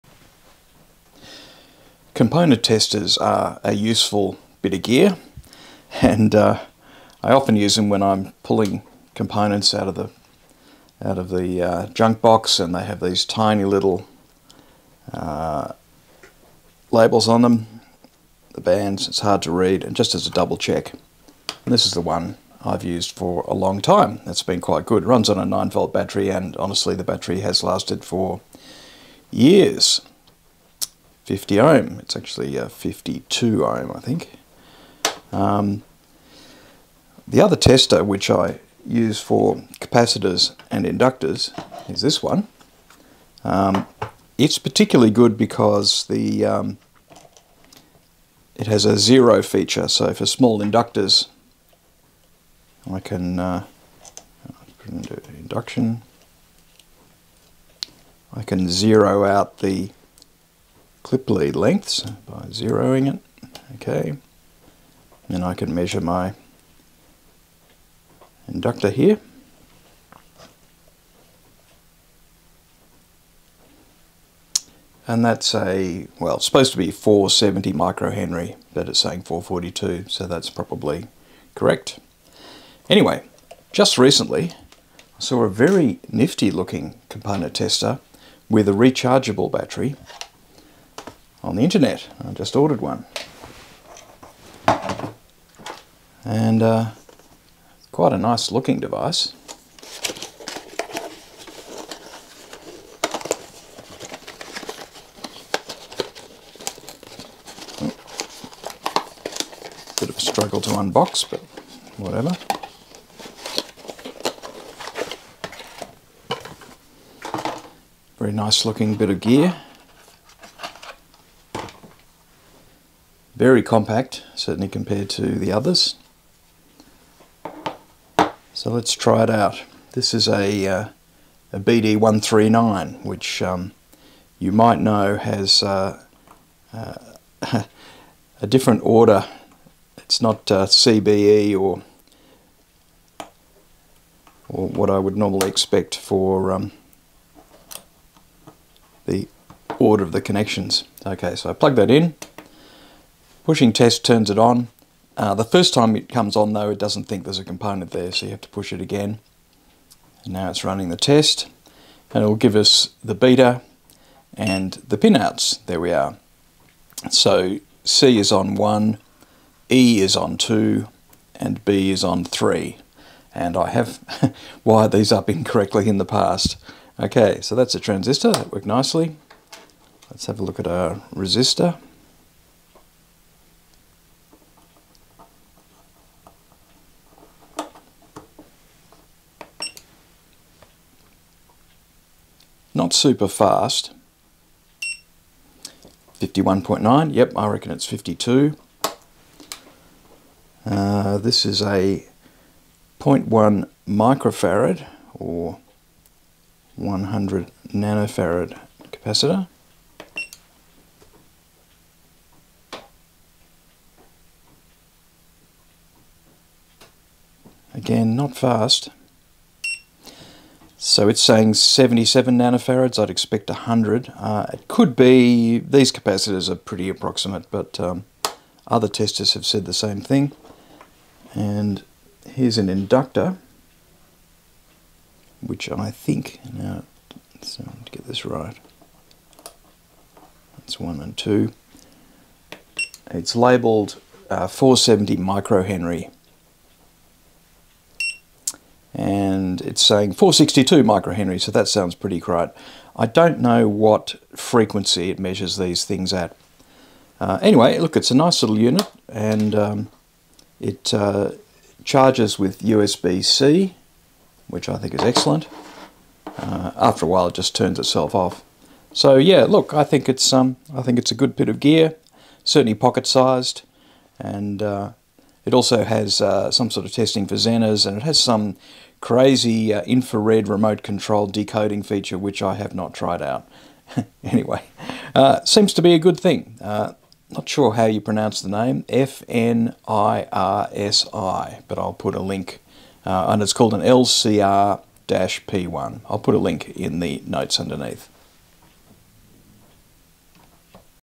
FNIRSI LCR-P1 Component tester
A quick walk through of a new component tester available from many places. I paid AU$44 in Australia but they are surely available cheaper direct from China. Seems well made and the USB-C rechargeable battery is a good feature.